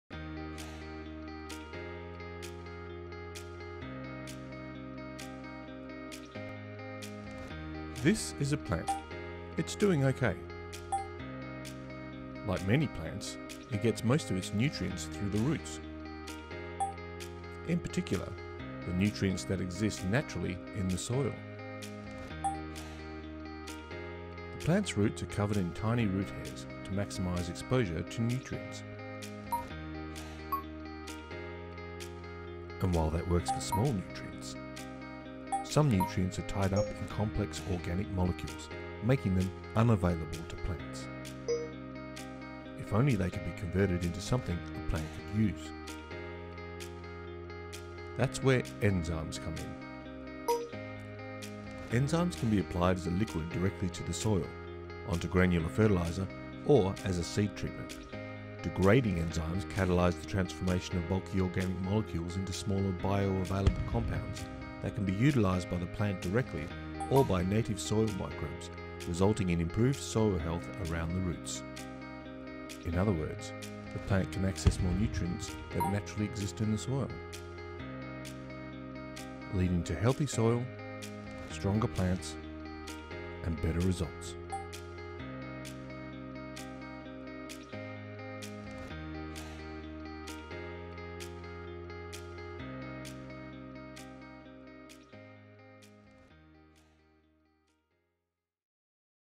Enzyme Explainer
Enzymes are proteins produced by all living organisms that act as a catalyst for chemical reactions.